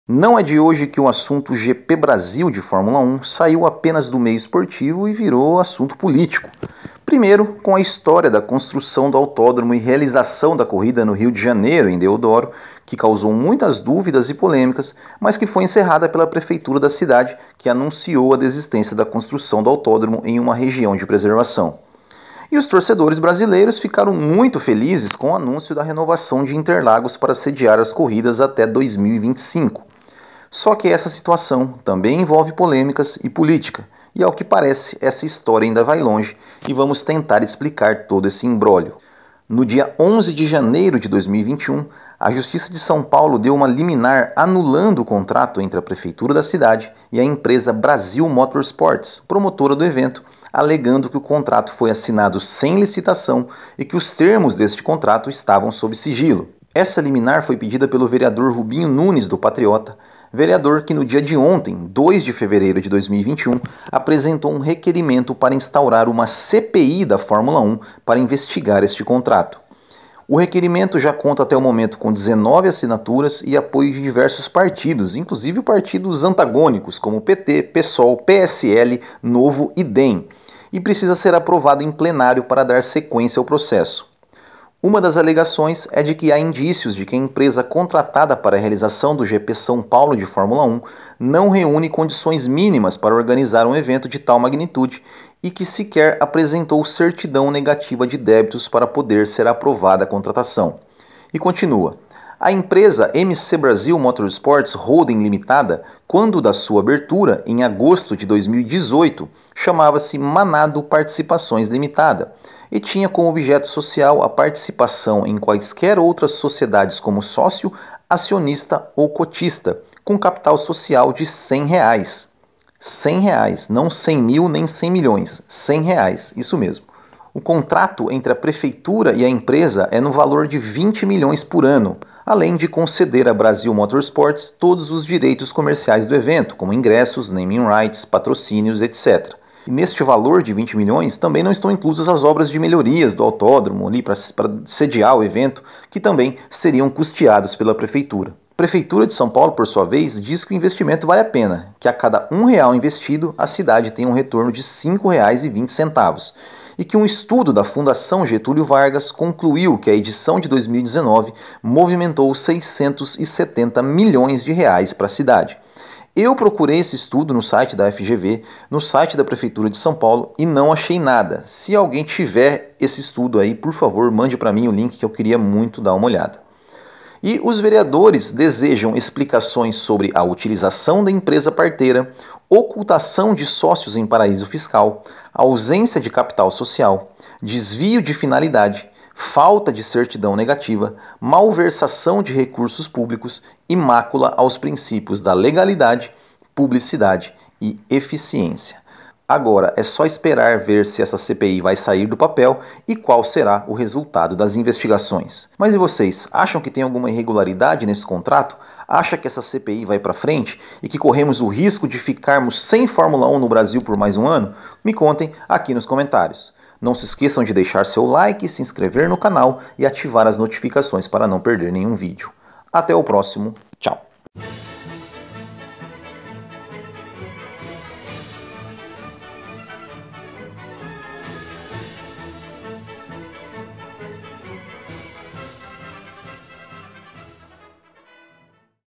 PLANTÃO! CPI da Fórmula 1 (GP BRASIL ameaçado?)
O GP BRASIL 2021 após ter seu contrato anulado por uma liminar no mês passado, agora pode ser alvo de uma CPI na cidade de São Paulo para investigar possíveis irregularidades no contrato entre a prefeitura e a Brasil Motorsports, promotora do evento. E nós fizemos um pequeno resumo dessa história